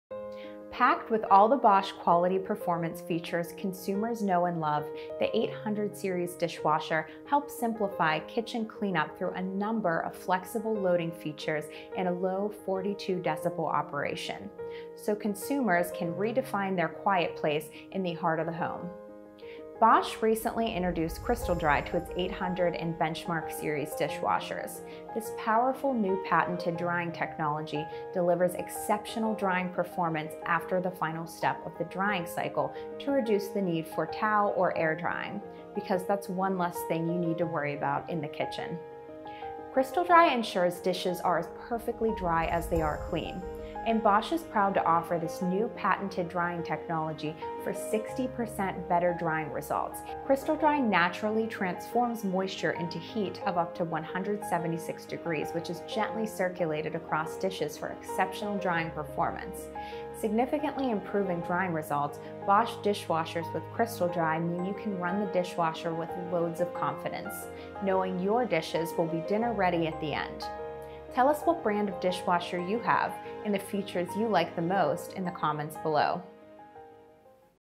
The Bosch 800 Series Dishwasher Overview
In the market for a new dishwasher?  Check out the functions and features of the Bosch 800 Series Dishwasher in this video!